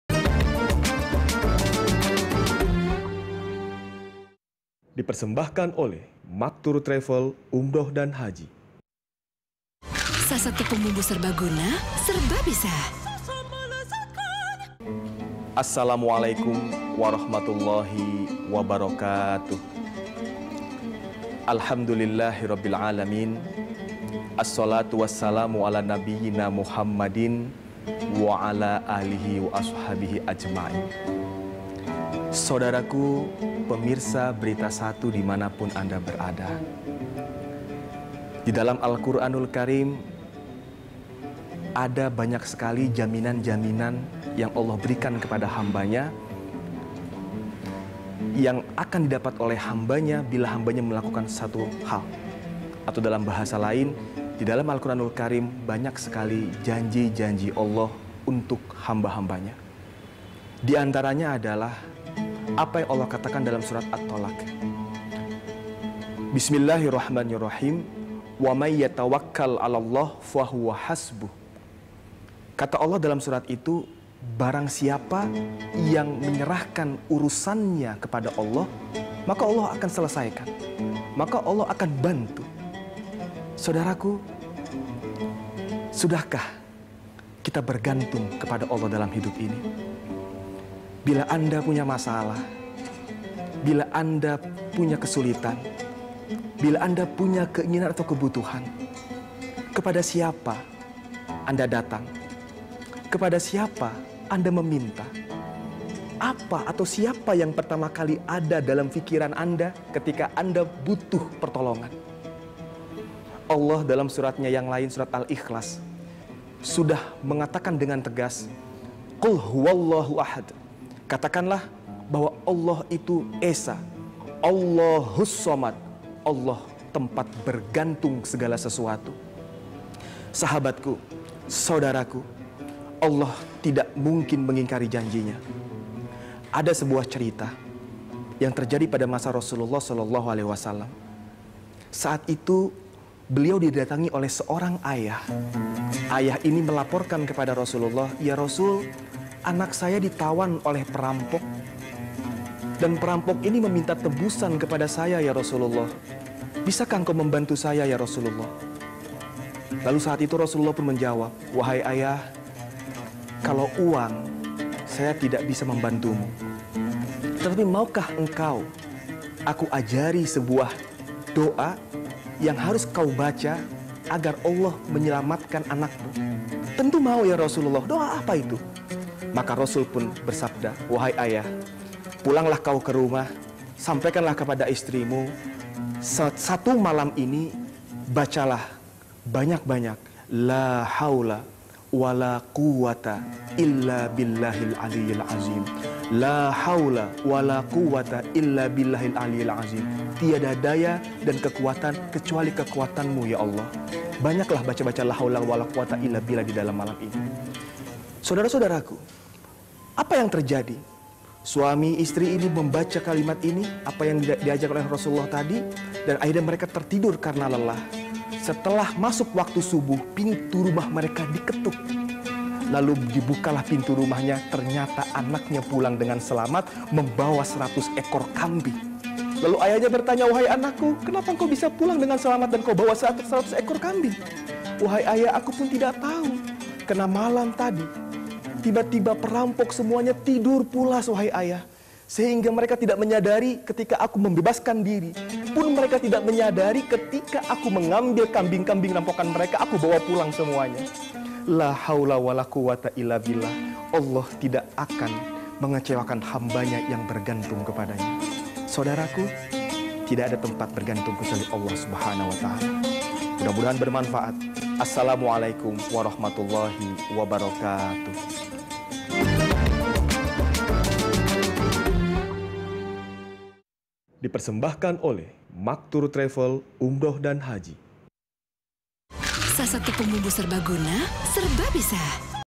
Kutipan Ramadan: Bergantunglah kepada Allah
"Allah berfirman, barangsiapa menyerahkan urusannya kepada Allah maka Allah akan selesaikan," jelas Ustaz Muhammad Hafidz Makmum

Facebook.com/BeritaSatu
Youtube.com/BeritaSatu